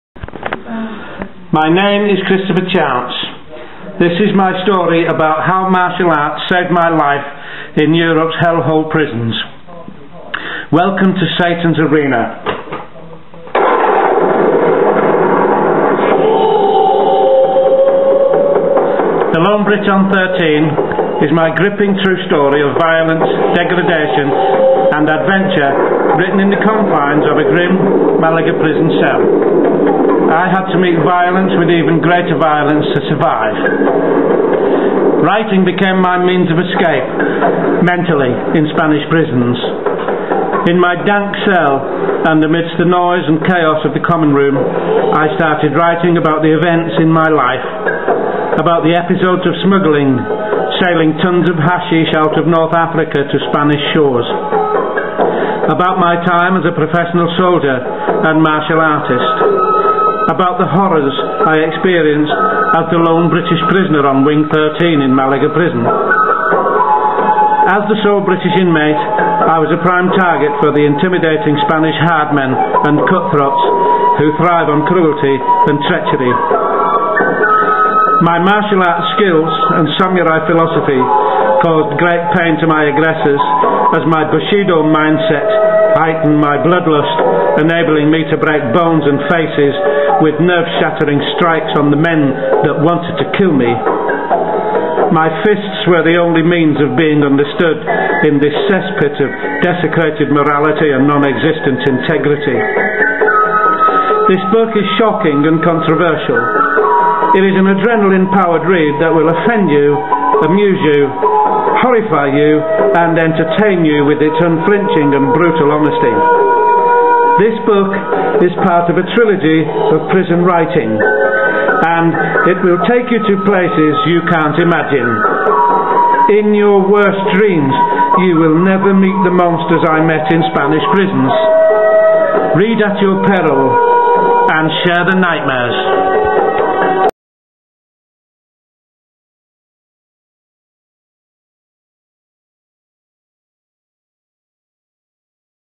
HARD HITTING TRUE-CRIME AUTHOR.
This is my first book, in a Trilogy, of my extreme violent life behind bars in Spains Notorious Prisons.